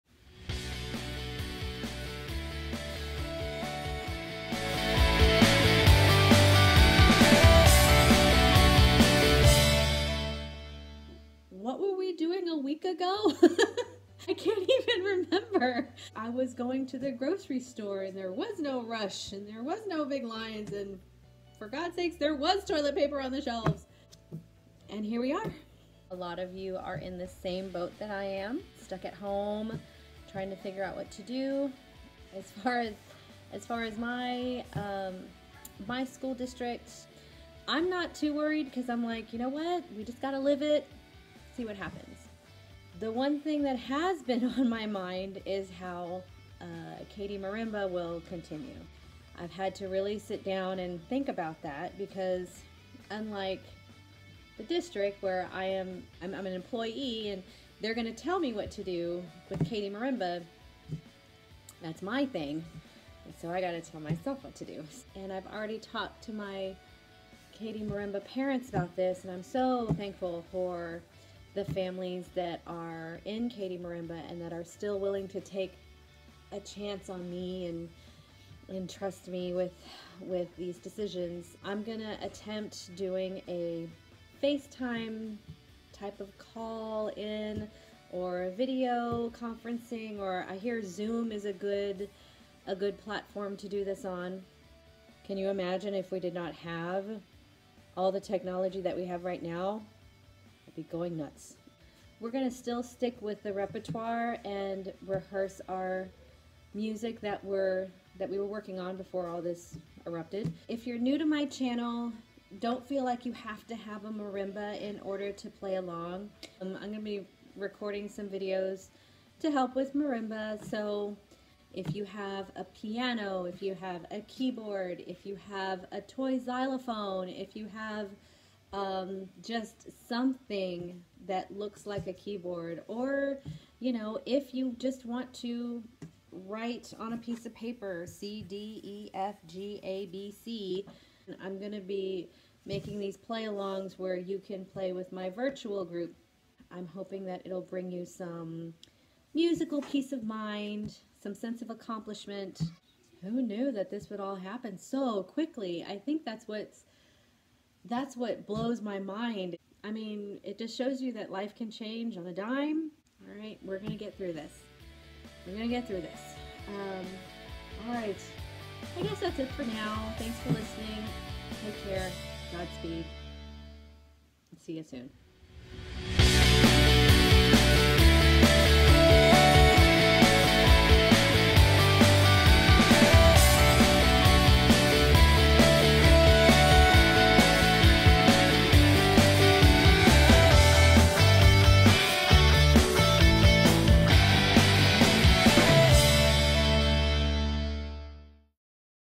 How will Katy Marimba continue?
Oh what times we are living in right now!  Things are so crazy right now, changing on a daily, sometimes hourly basis.  Well, it just goes to show that life changes in an instant--LITERALLY!!!  But I plan to carry on.  Find out how on this channel.  Stay positive, stay healthy, stay safe and WASH YOUR HANDS!